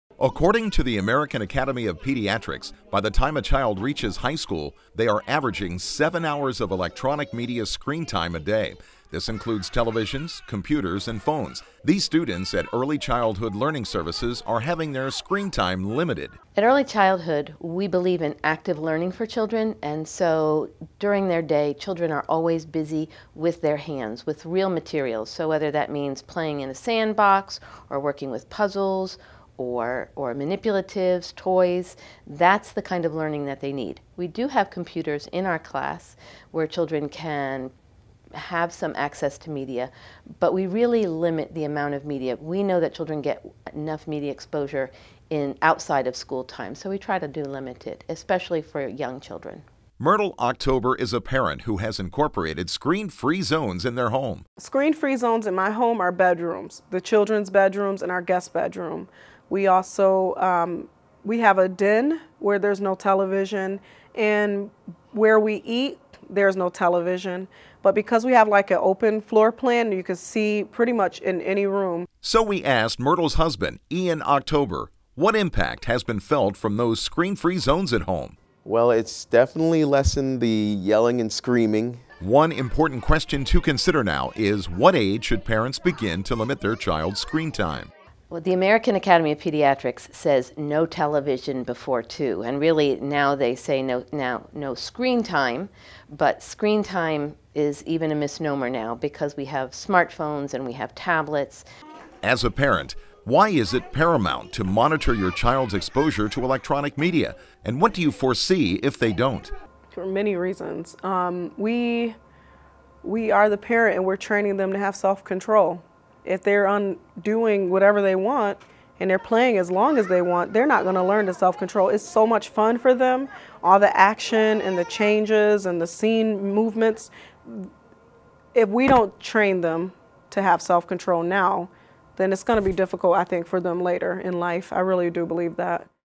Tots and Technology
According to the American Academy of Pediatrics, by the time a child reaches high school, they are averaging 7 hours of electronic media screen time a day. This includes televisions, computers and phones. However, students at Early Childhood Learning Services are having their screen time limited with hands-on activities.